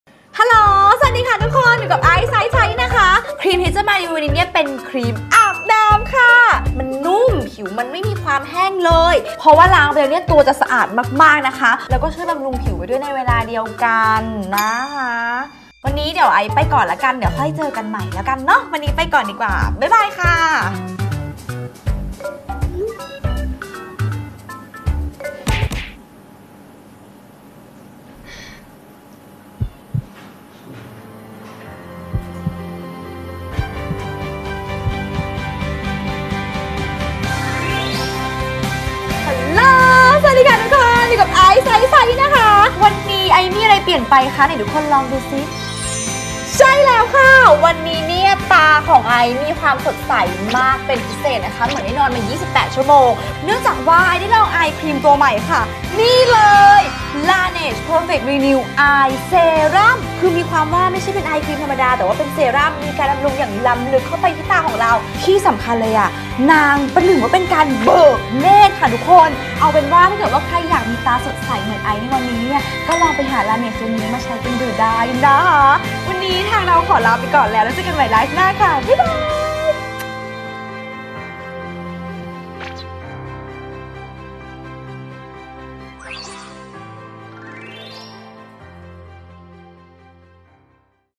LANEIGE Eyes Tell The Truth
จากเค้าโครงเรื่องจริง ดวงตาที่ไม่สดใส
อาจทำให้ใครคิดว่าไม่ได้พูดความจริง!
เพราะจริงหรือหลอก บอกได้ด้วยสายตา!!

ต่อหน้ากล้องดูสดใส ร่าเริง ใครจะรู้ว่าเบื้องหลังของสาว PADIE
เจออะไรบ้าง แล้วอะไรที่ทำให้เธอกล้าทำในสิ่งที่รักได้อย่างมั่นใจอีกครั้ง?!